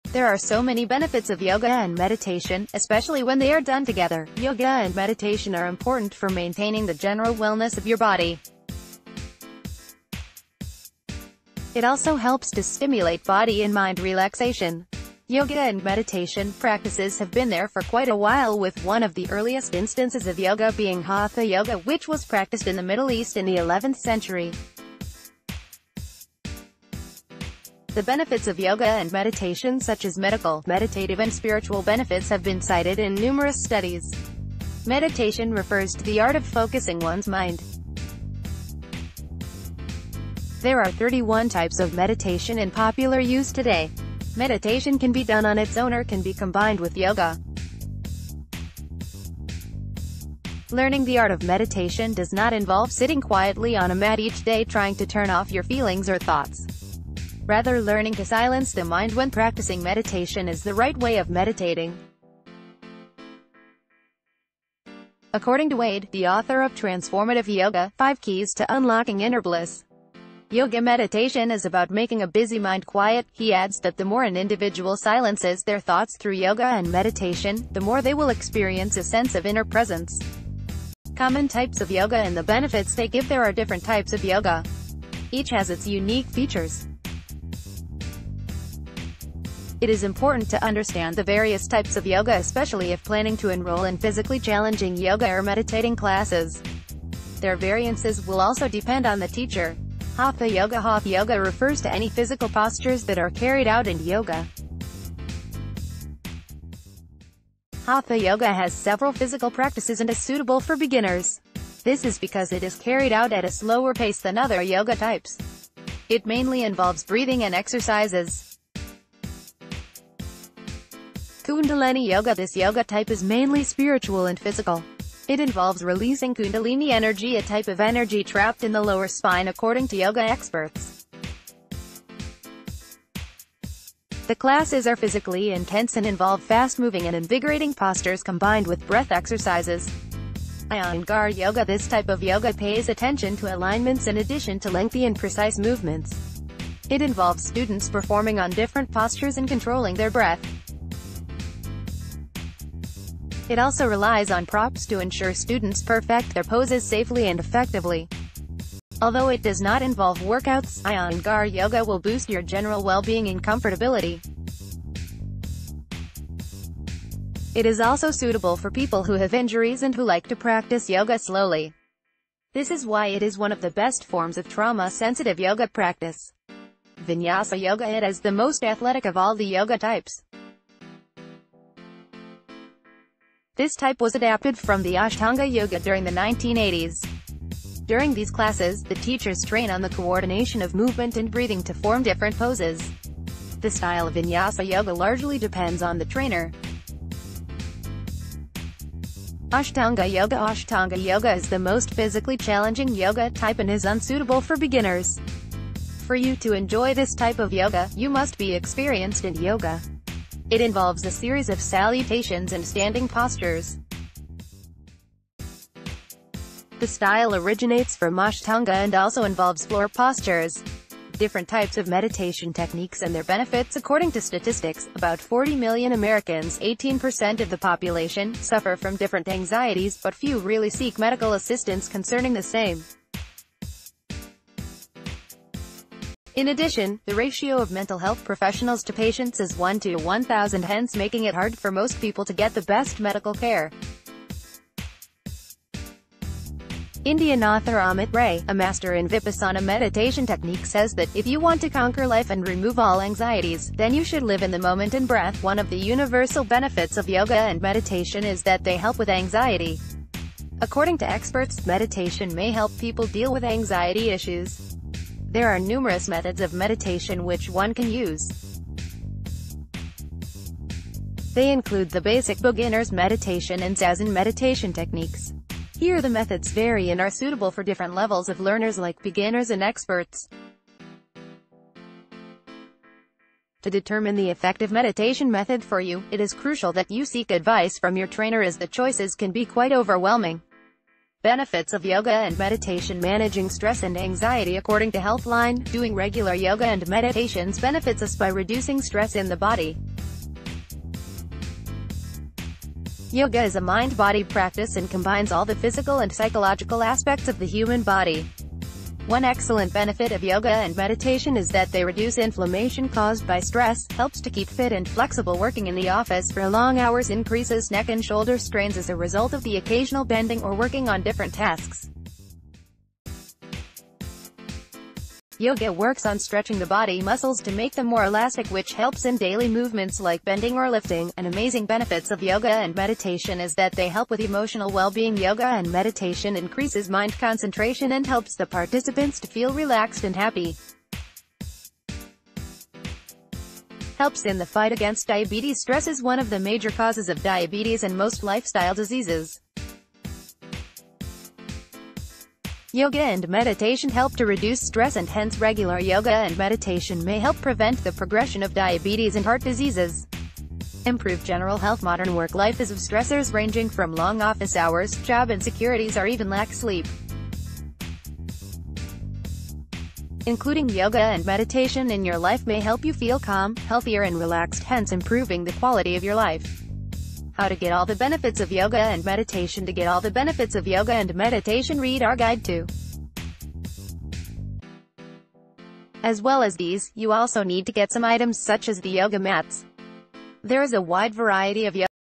These Benefits Of Yoga And Meditation Will Inspire You Today | Spirione Girl
There are so many benefits of yoga and meditation, especially when they are done together. Yoga and ...